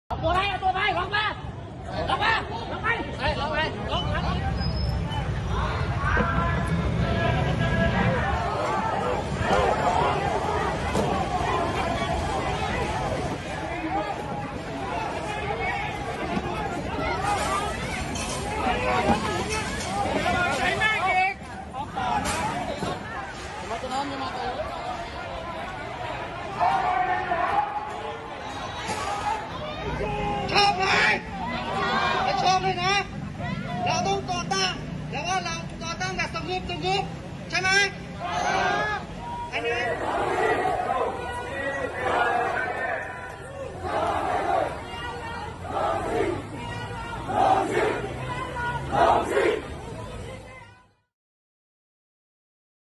JESHI LAIPINDUA SERIKALI, WANANCHI WAKASIRIKA, WAINGIA MTAANI KUANDAMANA....
Baada ya jeshi la nchi ya Myanmar kuipindua serikali wananchi wamejitokeza na kuandamana kupinga uamuzi huo.